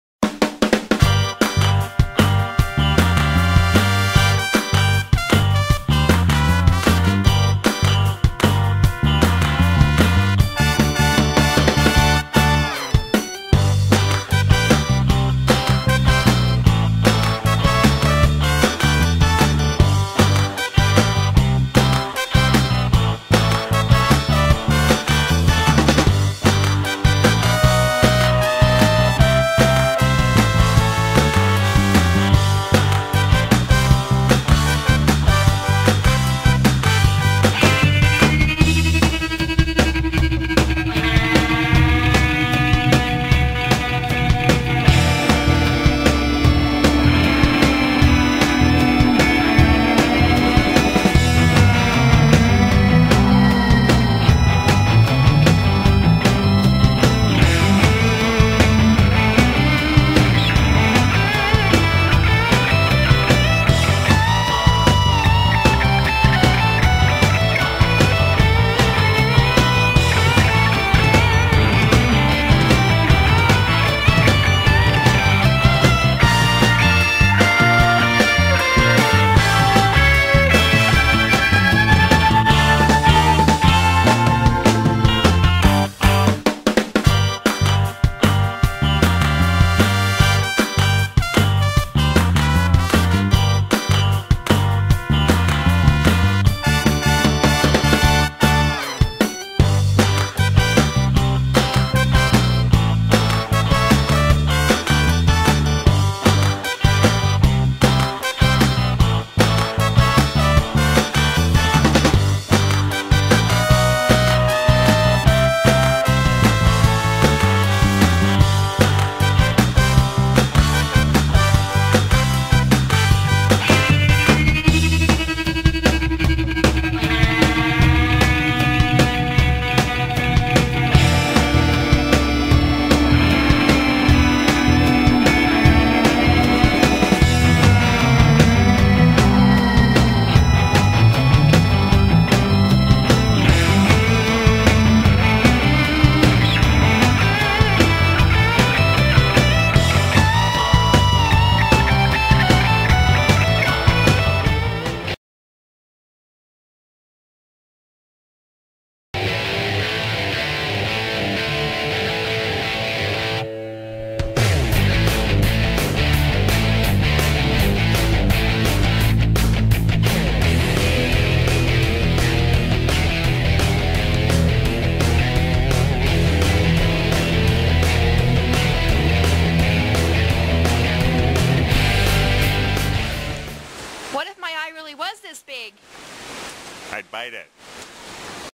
bathtub puppy
Simmy gets a bath in the sink